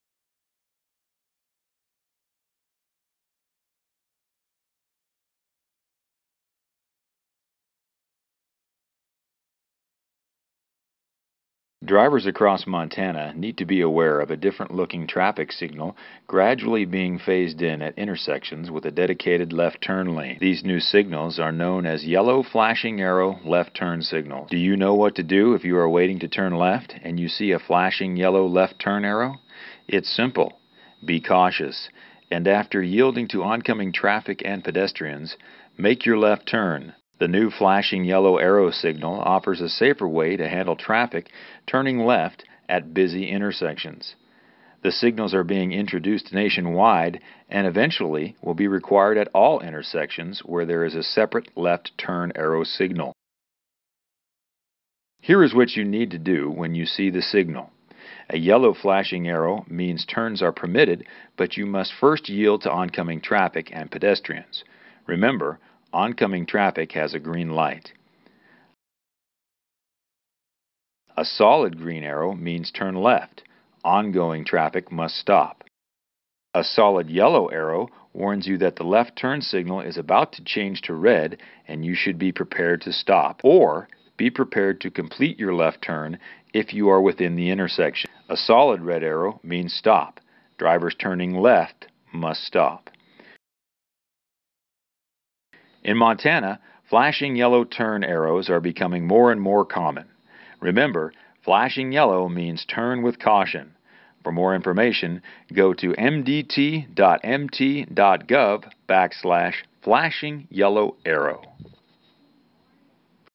Flashing Yellow Left-turn Arrows in Montana
The new flashing yellow arrow indication provides a more direct message than the circular green for turn lanes.  It reduces confusion since the turn lane display is no longer the same indication as what is displayed for the adjacent through lanes.